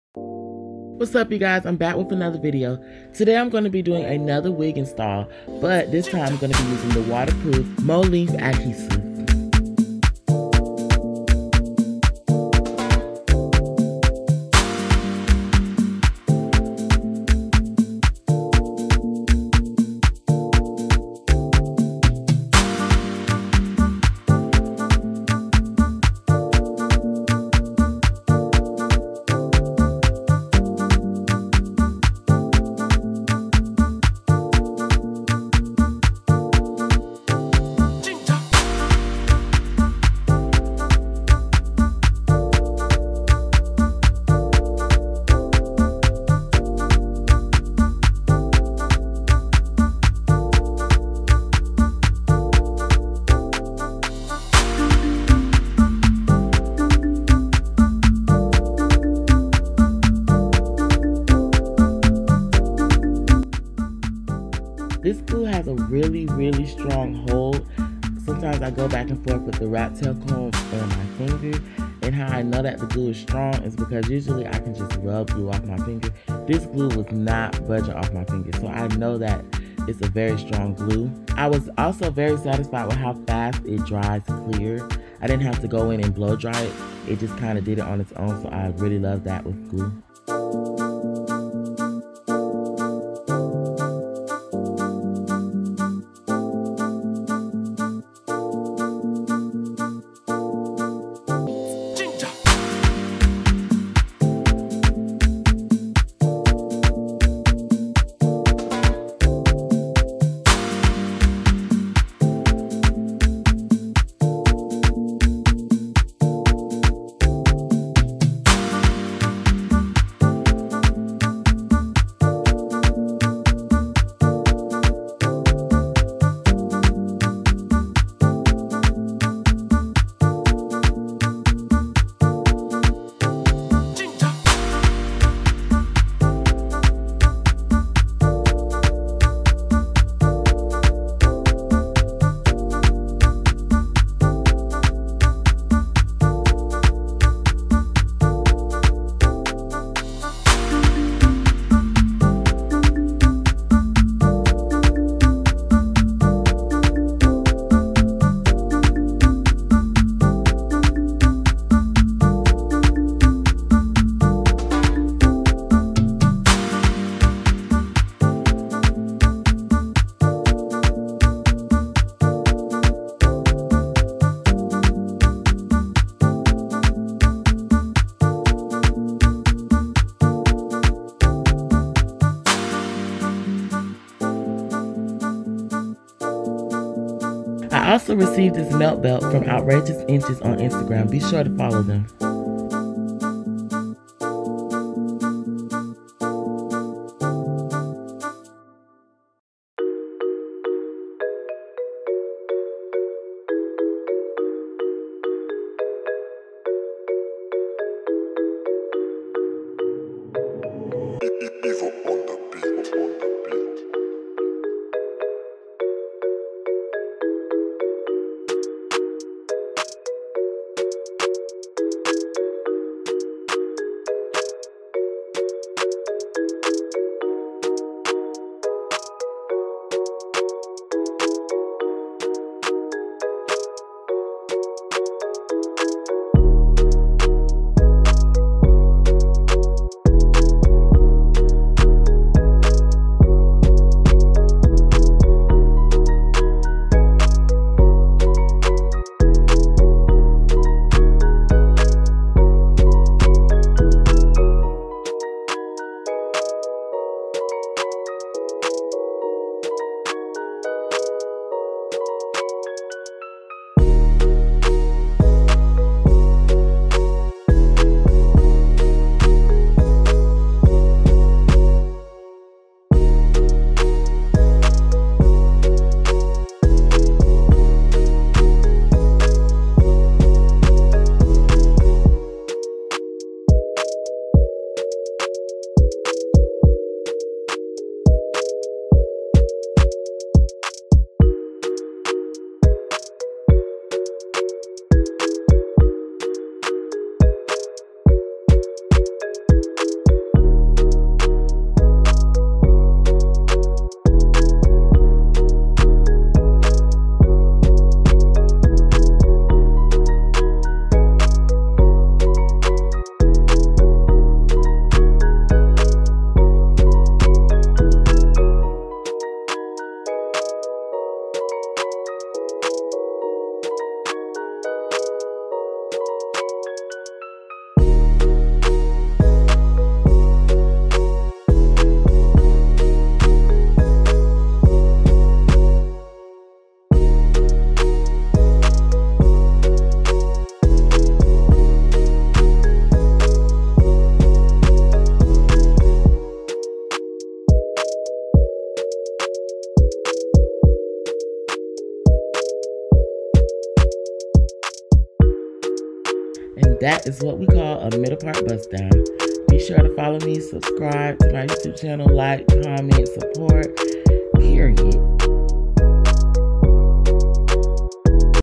Frontal Wig Install using Mo Length Lace Adhesive!
Welcome to SBMTV!

In this video I’ll be demonstrating another frontal wig install but this time ill be using Mo Length adhesive and giving my honest opinion!!  comment below and let me know what type of content you guys want to see more of on my channel!